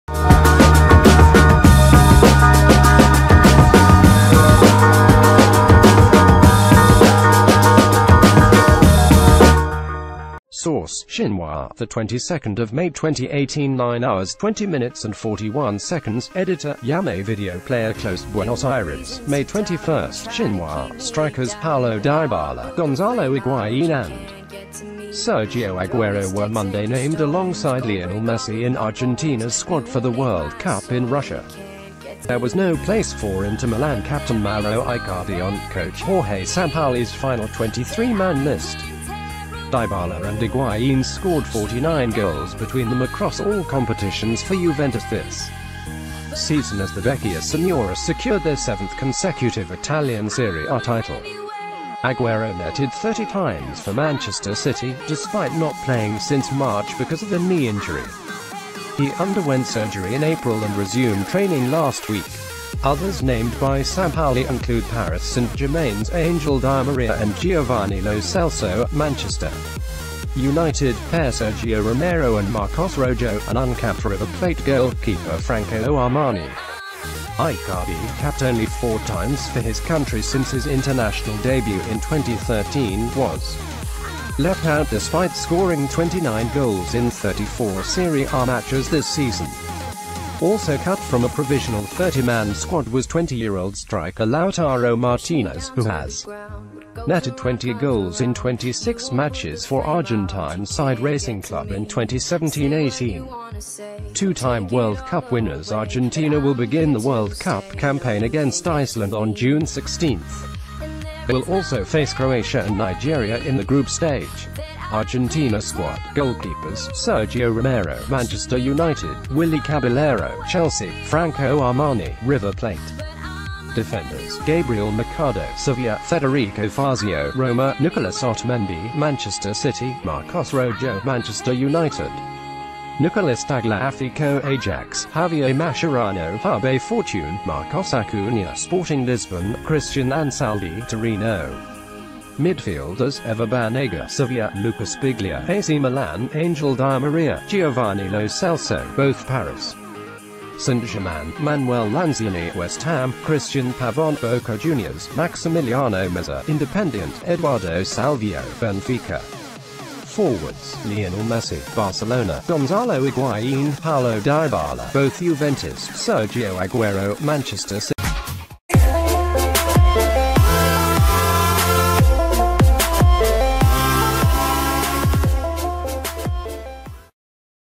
Dybala, Higuain join Messi in Argentina World Cup squad
► Source: Xinhua| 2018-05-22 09:20:41|Editor: Yamei Video PlayerClose BUENOS AIRES, May 21 (Xinhua) --...